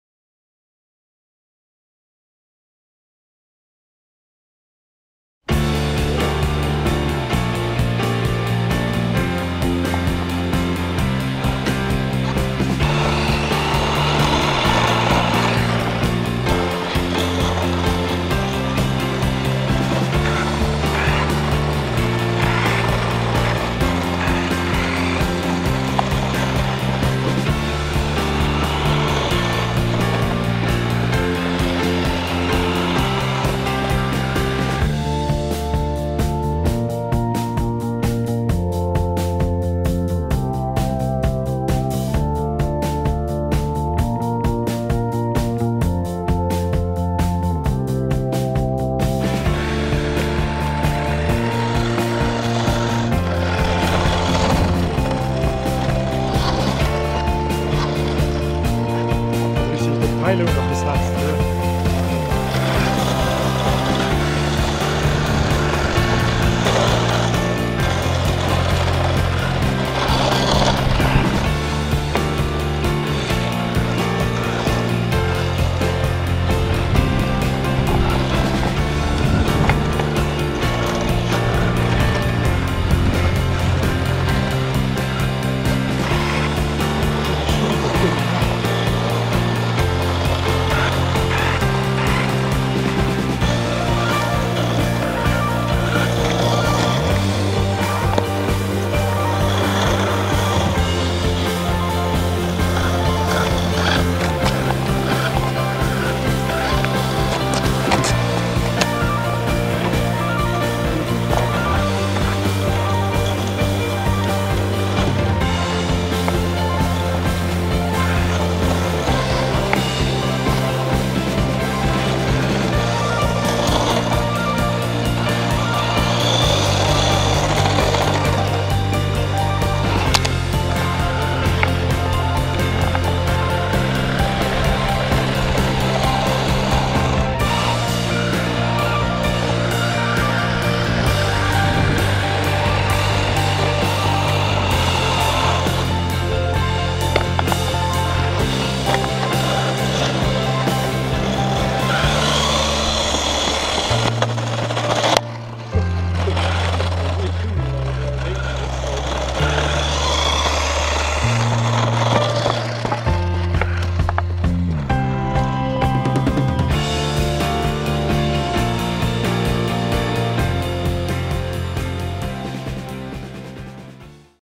GoPro // Lamborghini Huracan von Maisto in 1/14 / RC Car Testfahrt auf Asphalt
Hier das ferngesteuert Hurcan als Onroadcar auf dem Asphalt gefahren. Für die Wohnung ist der definitiv zu schnell. Allerdings hat er keine proportionale Lenkung. Der Lenkeinschlag ist immer voll nach rechts oder links. Bei hoher Geschwindigkeit etwas schwierig. Aber sonst ein tolles Modell.
Here the remotely Hurcan down on the asphalt as Onroadcar. For the apartment is definitely too fast. However, he has no proportional steering. The steering angle is always full to the right or left. At high speed it somewhat difficult. But otherwise a great model.